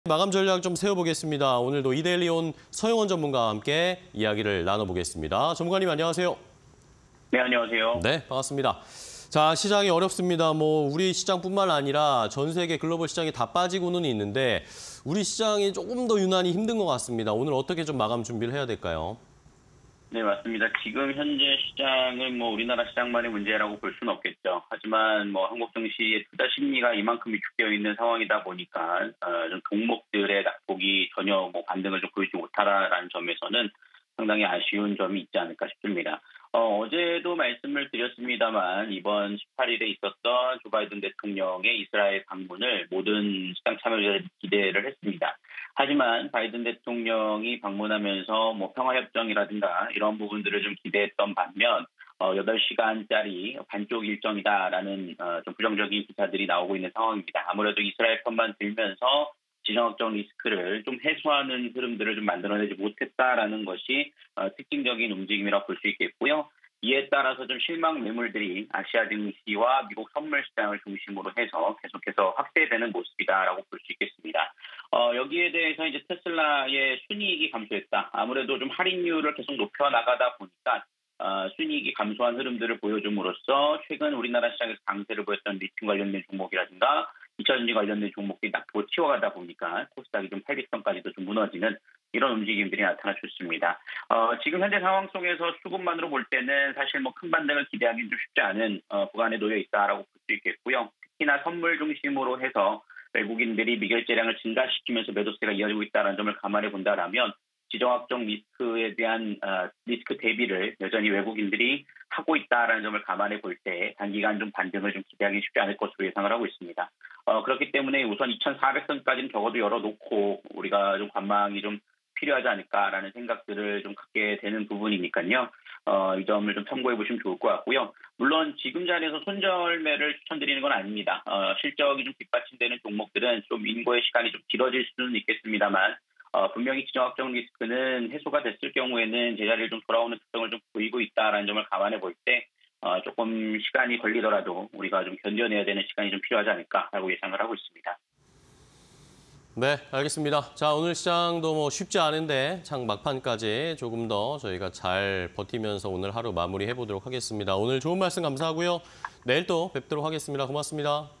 중동 위기감 지속… 코스피 2,400선 위협_서용원의 마감 전략 (20231019)
- 코스피, 지정학적 리스크부각에 약세
- 2차전지‧반도체 업종 약세
- 중동 위기감 지속… 코스피 2,400선 위협